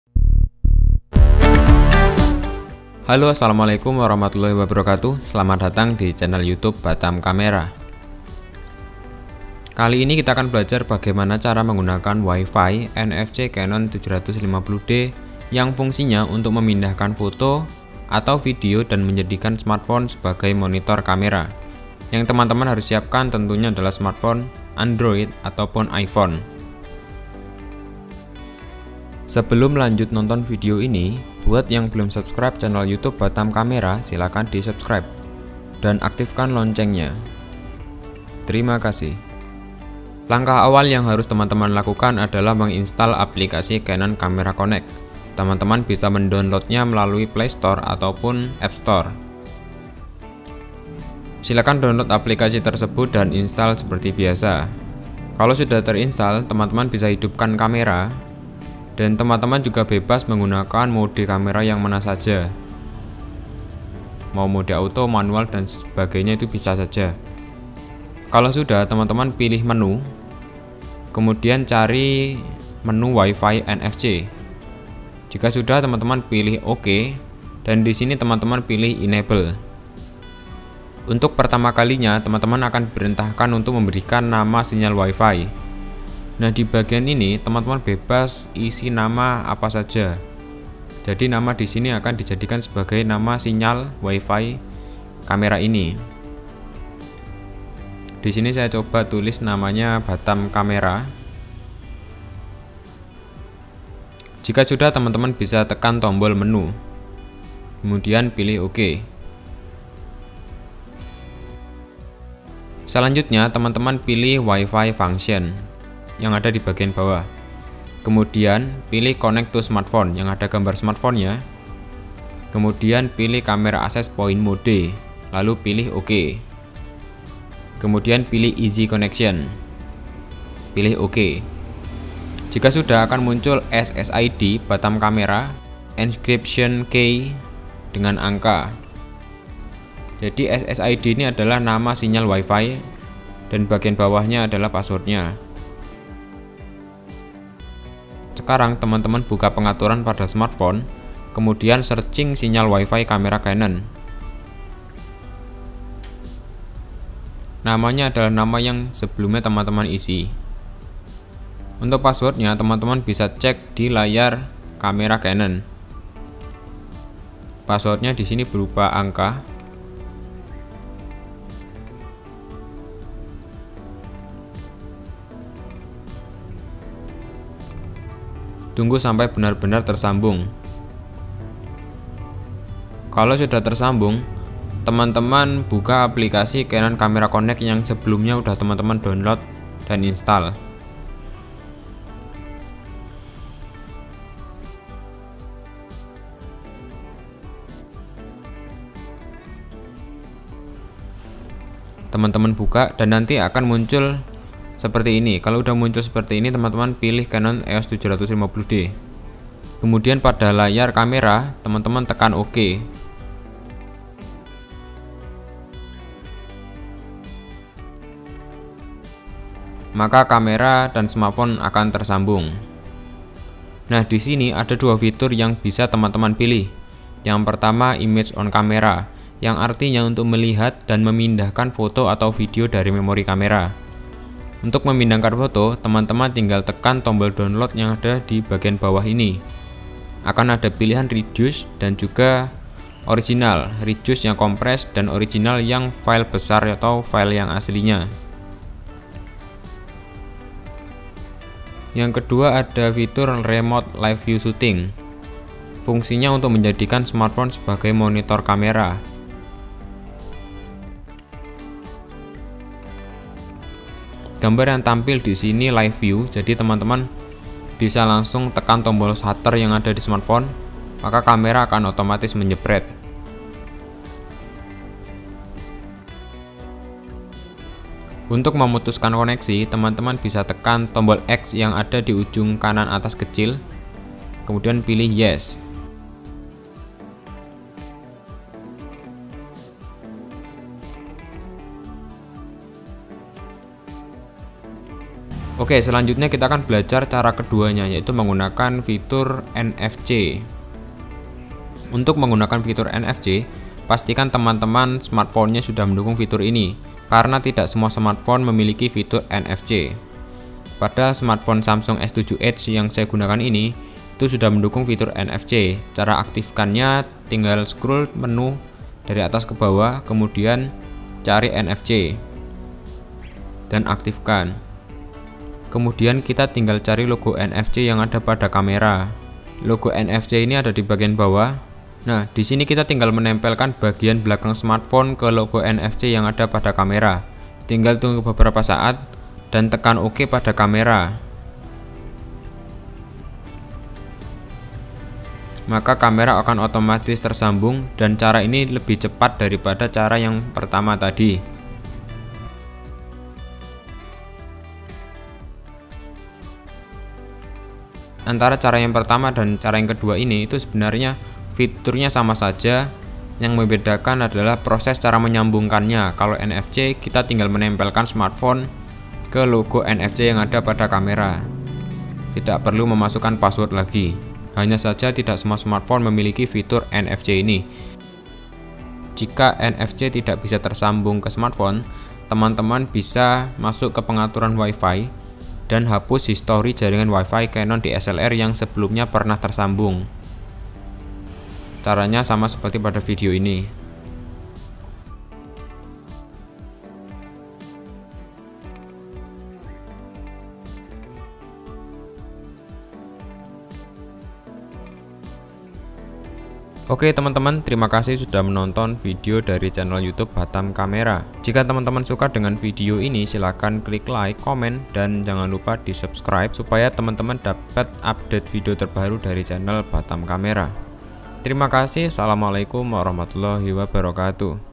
Cara Menggunakan WIFI/NFC Kamera Canon 750D Untuk Pindah Foto Video Dan Remote Video
Buat teman-teman yang bingung cara gunain fitur Wifi atau NFC, silakan tonton video ini sampai selesai, lengkap dengan panduan pake NFCnya yang lebih cepat dibanding fitur Wifi biasanya.

Fitur Wifi/NFC ini gunanya untuk mempermudah kita saat mau memindahkan foto/video ke smartphone, tinggal buka aplikasi dan tekan download maka foto/video akan langsung berpindah ke smartphone.

Cara ini pada umumnya berlaku untuk semua tipe kamera canon 750D, 760D, 80D, 70D, dan sejenisnya, mungkin hanya ada sedikit perbedaan pada tombol, dan posisi nfc nya.